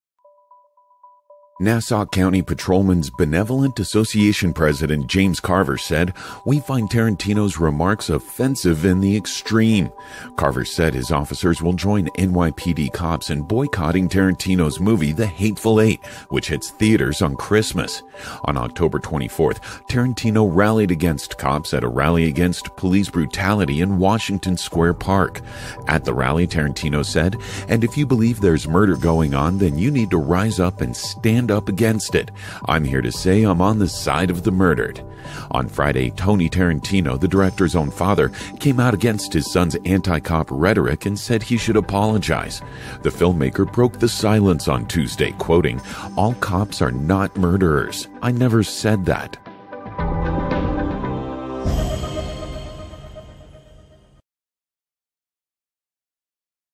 Nassau County Police Join NYPD in Boycotting Tarantino Film ‘The Hateful Eight’
As far as the police are concerned, director Quentin Tarantino is still in the reservoir dog house. Nassau county cops joined the NYPD’s boycott of the “Pulp Fiction” director’s upcoming movie for his anti-police rants.